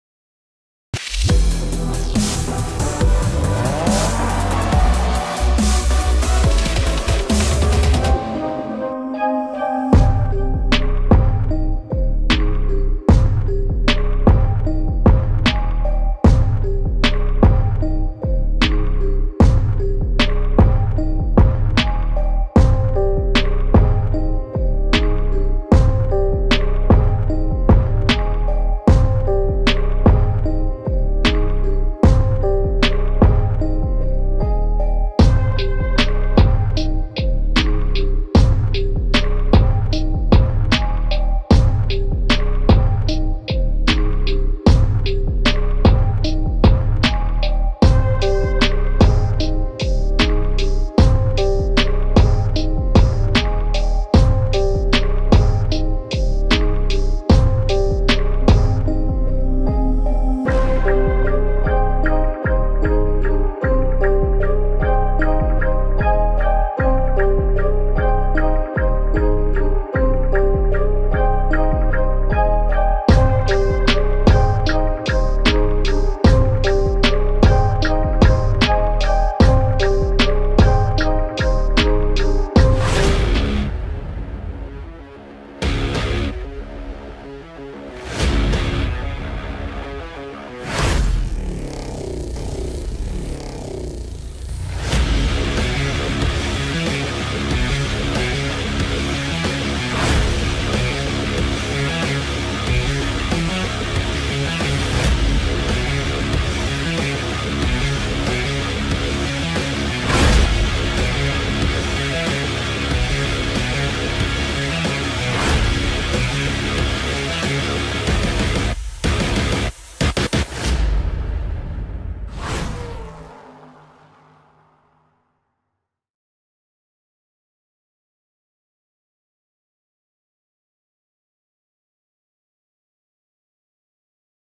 Bus Ruta San Miguel / Mr. De Leon
Pequeño video clip del bus de la ruta San Migual - La Doña en el car show fest 2022 realizado en el estadio Rommel Fernandez.